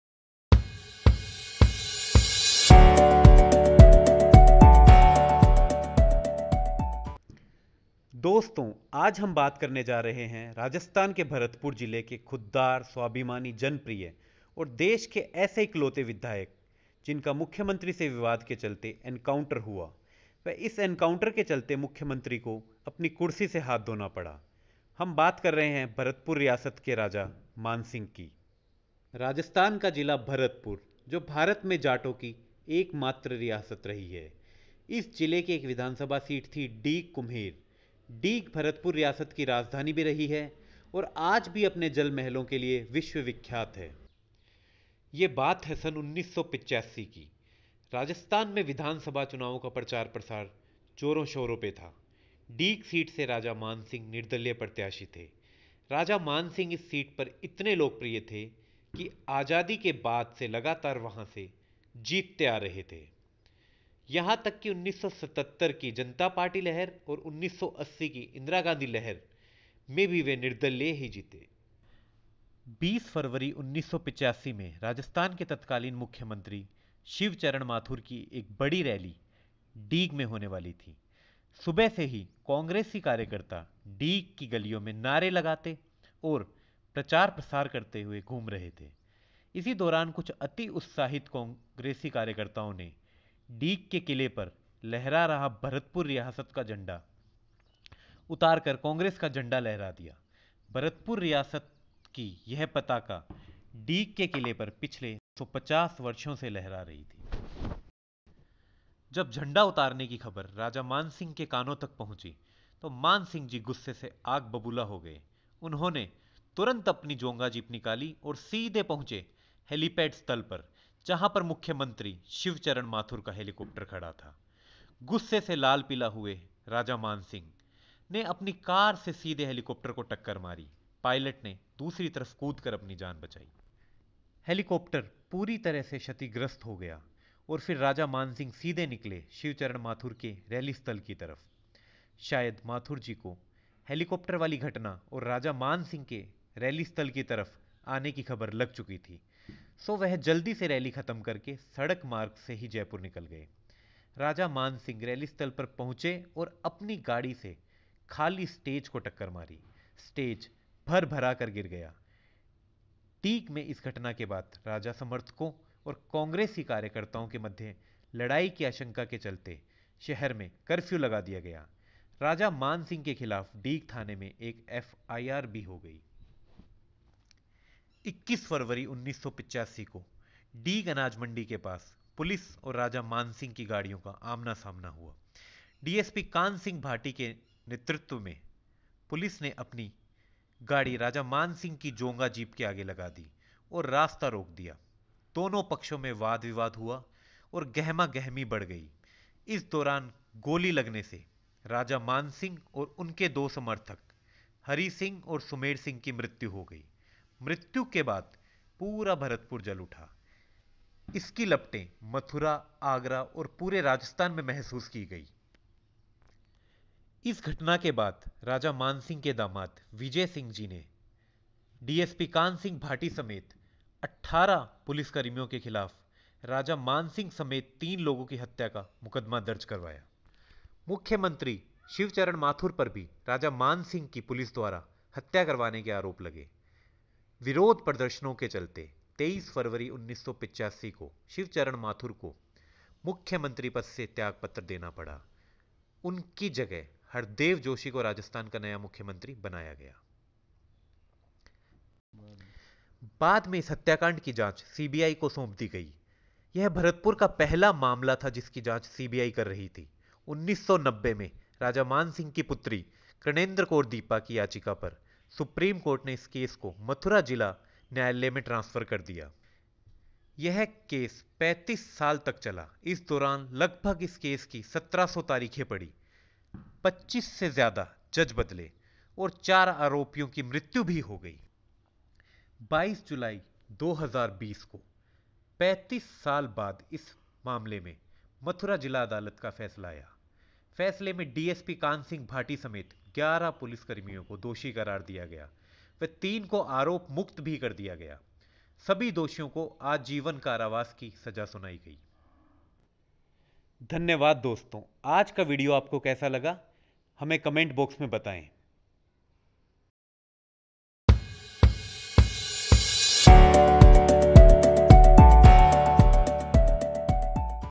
Bharatpur| Jat Raja Maan Singh encounter case | भरतपुर | राजस्थान
ये वीडियो राजस्थान के भरतपुर रियासत के राजा मानसिंह के एनकाउंटर और उसके बाद चले केस की घटना पे आधारित है | इसका केस का  फैसला 22 जुलाई 2020 को मथुरा न्यायलय ने सुनाया है| इस वीडियो के द्वारा जानिए इस केस की पूरी कहानी|

Team(The Shiksha Circle):
Devendra Dhaked
Sudhir Sharma
Yatish Bedi